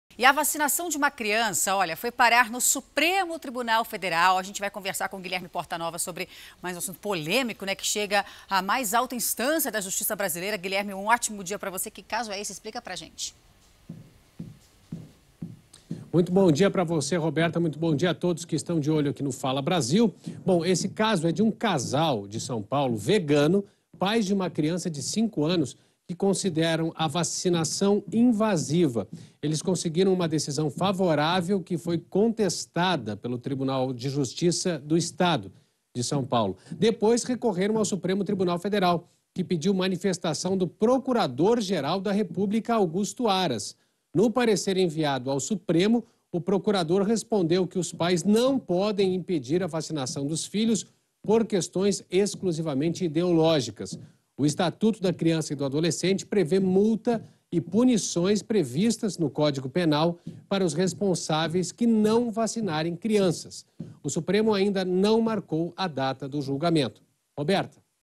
Supremo Tribunal Federal irá julgar obrigatoriedade de vacinação em crianças
Um casal de São Paulo, vegano, pais de uma criança de cinco anos, consideram a vacinação invasiva. Eles conseguiram uma decisão favorável que foi contestada pelo Tribunal de Justiça do estado. Depois, recorreram ao Supremo Tribunal Federal que pediu manifestação do Procurador-Geral da República, Augusto Aras. No parecer enviado ao Supremo, o Procurador respondeu que os pais não podem impedir a vacinação dos filhos por questões exclusivamente ideológicas. O Estatuto da Criança e do Adolescente prevê multa e punições, previstas no Código Penal, para os responsáveis que não vacinarem crianças. O caso ainda não tem uma definição.